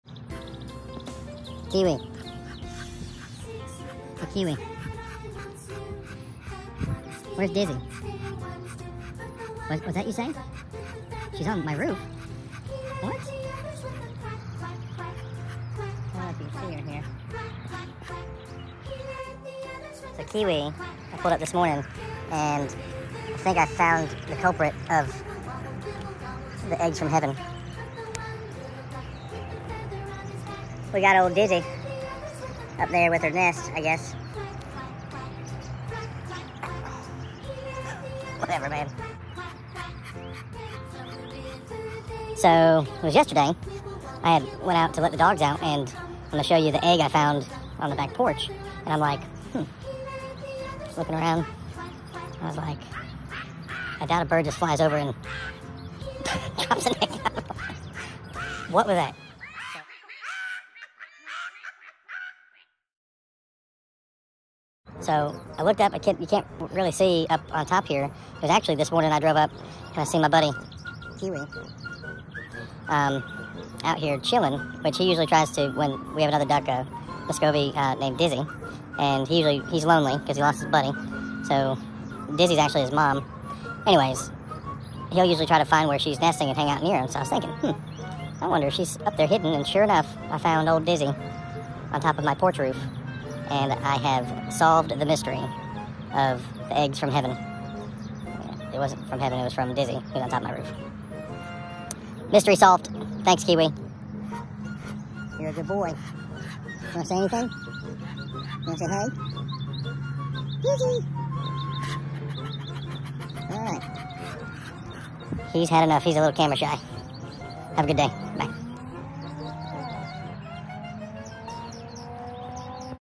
The Mystery Eggs From Heaven
Come with me as I solve the Mystery of how an egg found its way into my back yard .😳😳🤔🤔🤔🤪🤪